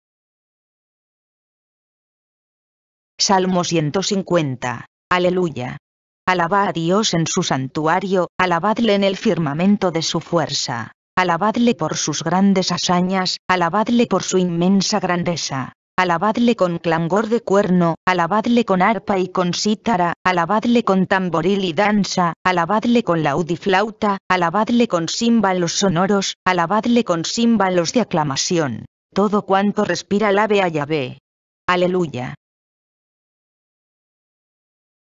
LA SANTA BIBLIA,VERSIÓN BIBLIA DE JERUSALÉN 1976, Salmos 150
Salmo 150

¡Aleluya!
 Alabad a Dios en su santuario, alabadle en el firmamento de su
fuerza,
 alabadle por sus grandes hazañas, alabadle por su inmensa grandeza.
 Alabadle con clangor de cuerno, alabadle con arpa y con cítara,
 alabadle con tamboril y danza, alabadle con laúd y flauta,
 alabadle con címbalos sonoros, alabadle con címbalos de
aclamación.
 ¡Todo cuanto respira alabe a Yahveh! ¡Aleluya!